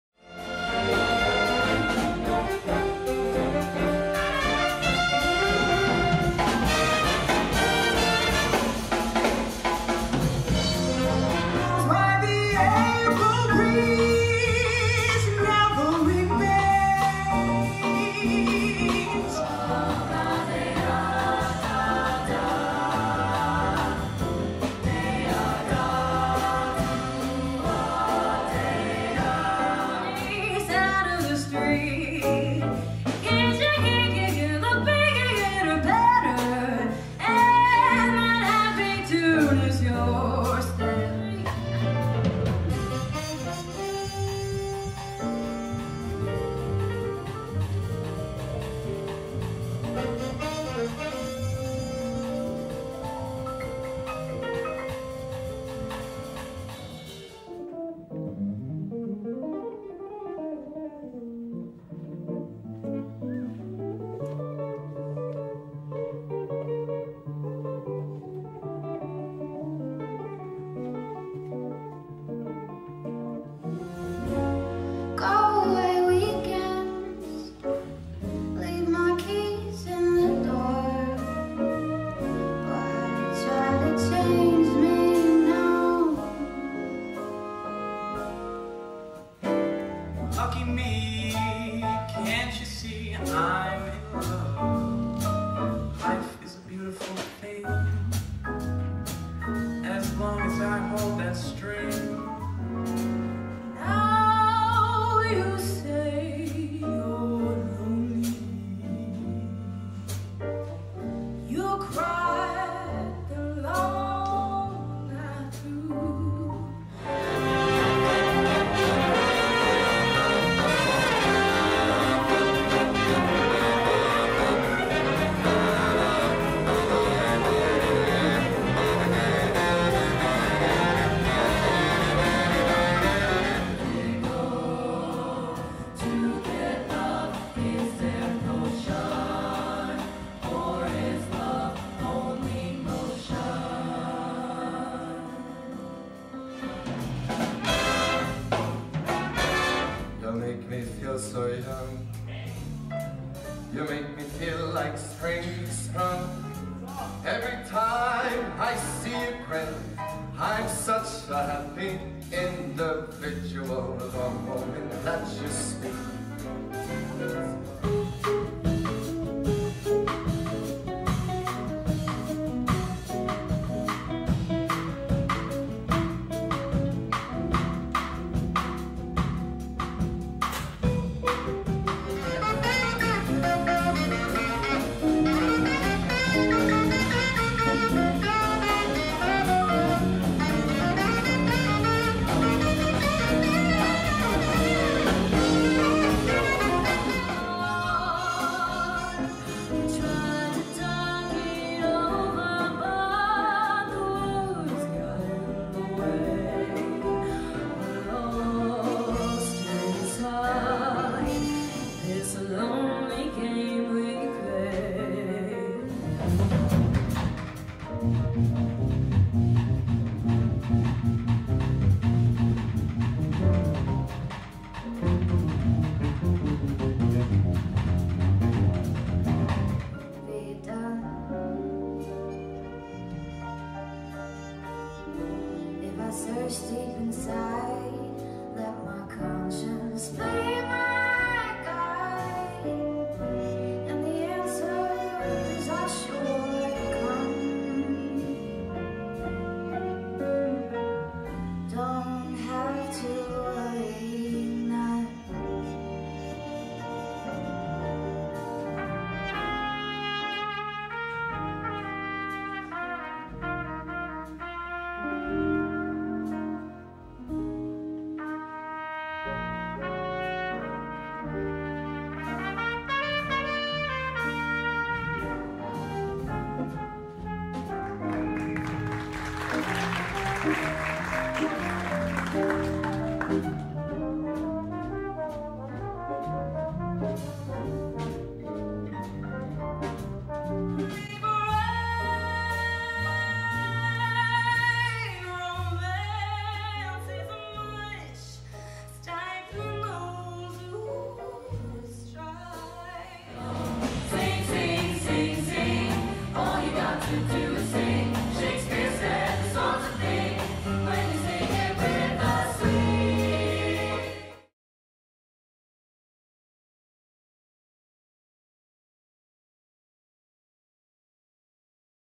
Five Towns College "Evening of Jazz" 2018 - Short version
Highlights from the May, 2018 "Evening of Jazz" concert - featuring the exceptional music students of Five Towns College.
There is a longer version (11 minutes) available as well.